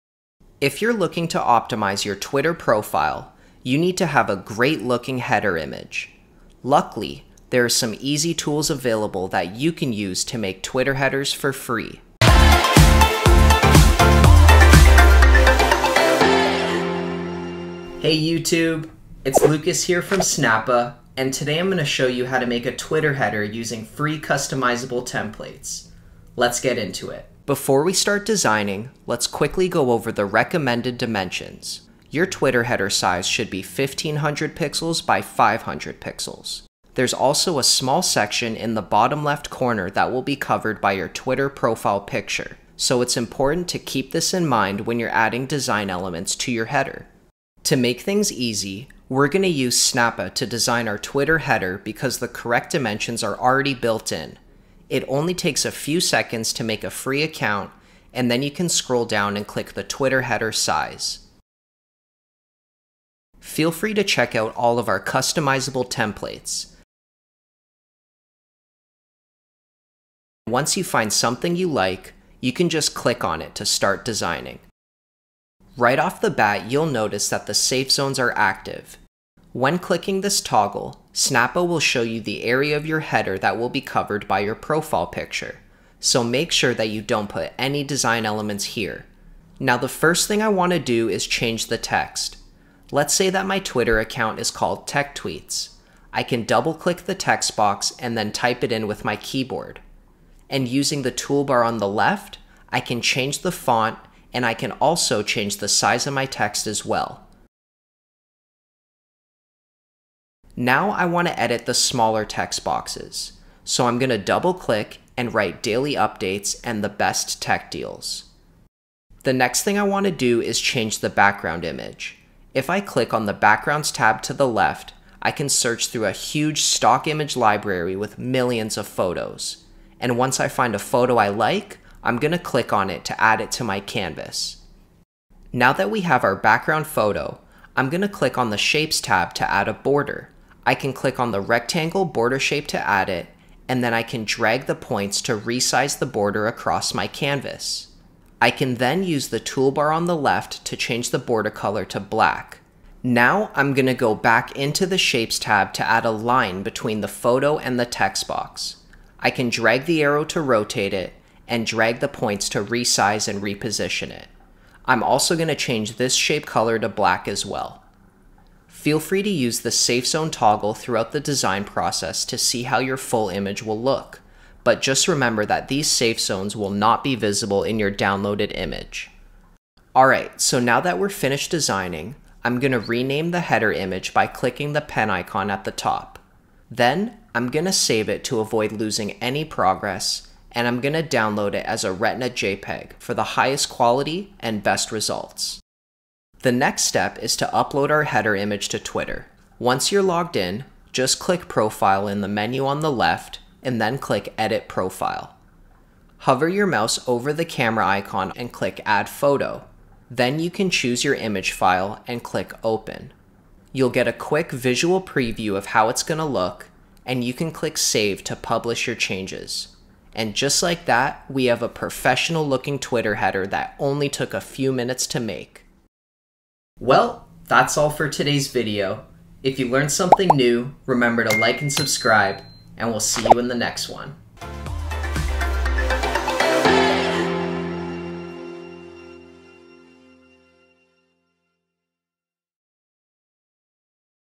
How to Make an X (Twitter) Header in Snappa + Free Templates
In this video, I show you how to make an X (Twitter) banner for free using customizable templates.

0:00 Intro
0:27 The Best Twitter Header Size
0:47 Twitter Header Templates
1:15 How to Design a Twitter Header
3:16 How to Upload a Banner to Twitter
3:47 Outro